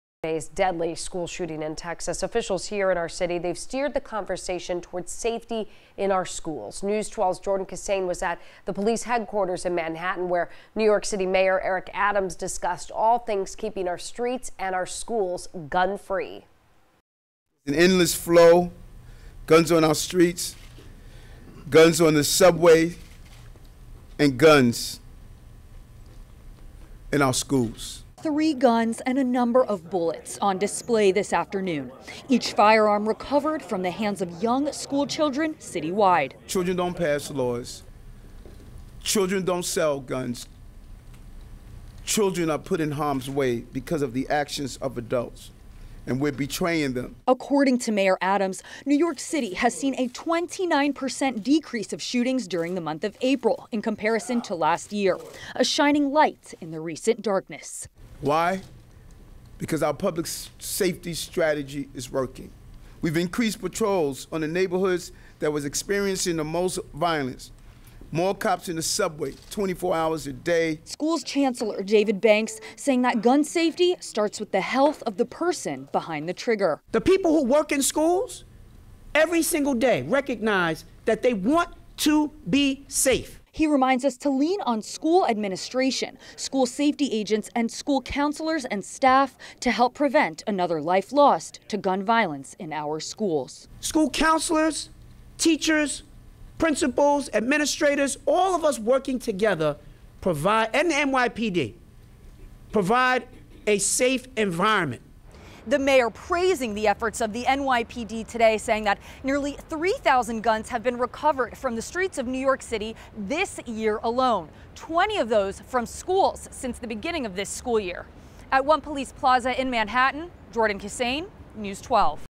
Mayor Adams discusses enhanced public safety in aftermath of Texas school shooting
Adams made it clear his objective is keeping the city’s streets and schools gun free.